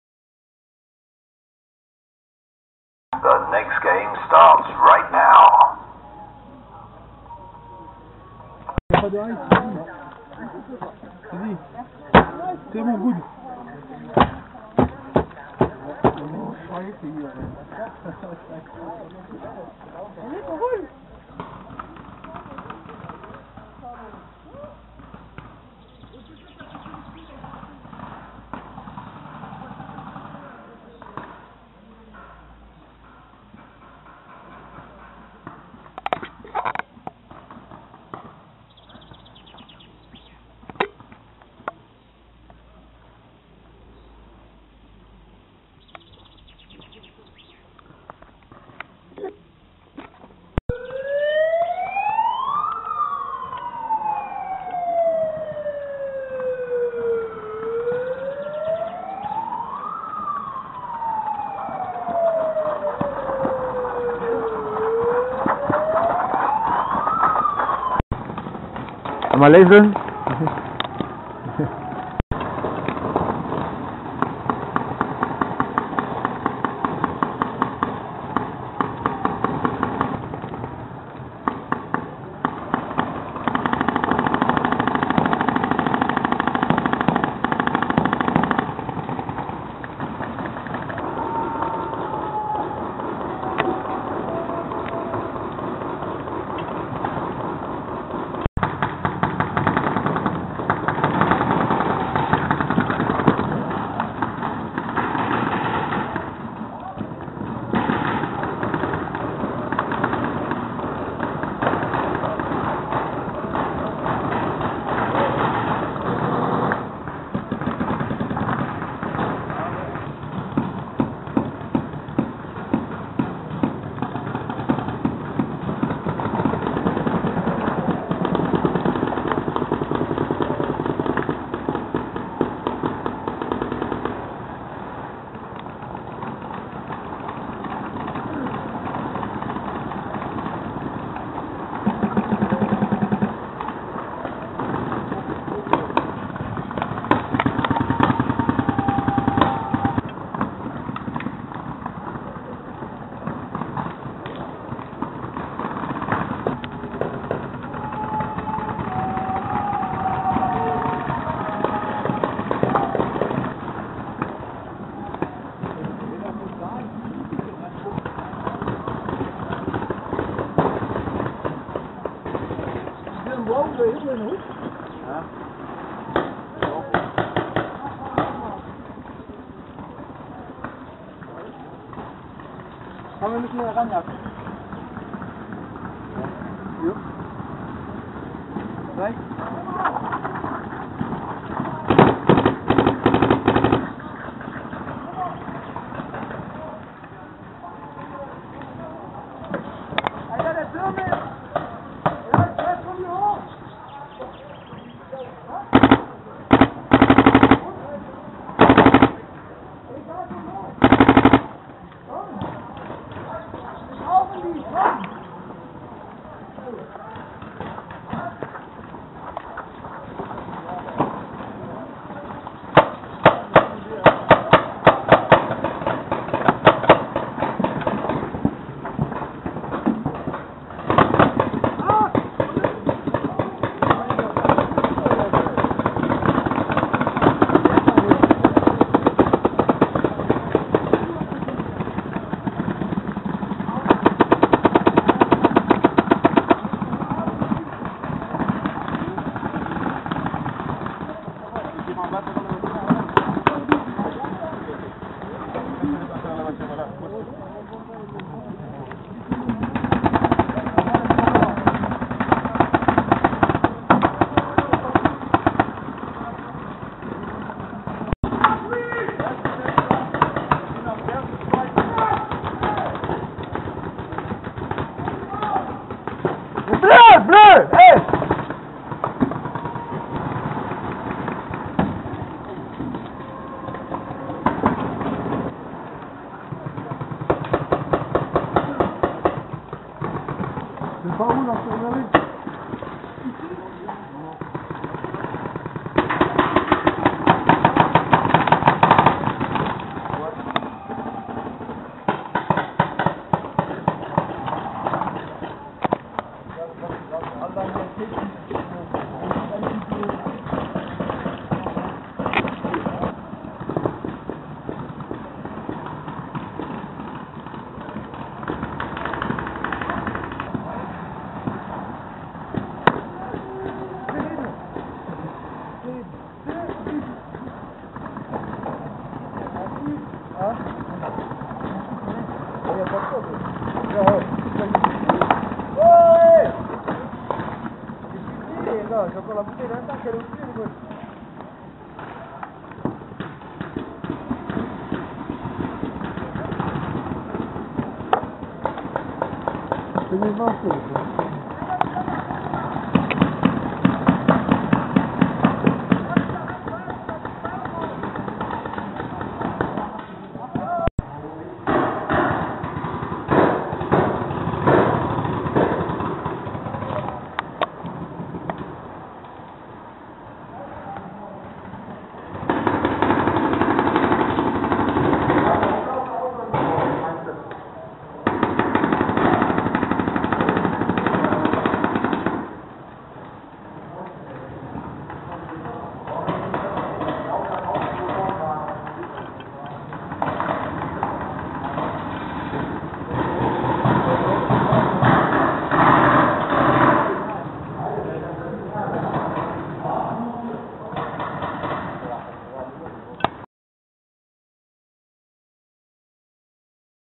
Veckring Big Game 2k16
Paintballevent Big Game in Veckring/F.
Das Big Game findet in der alten Kaserne aus dem 2. WW. statt.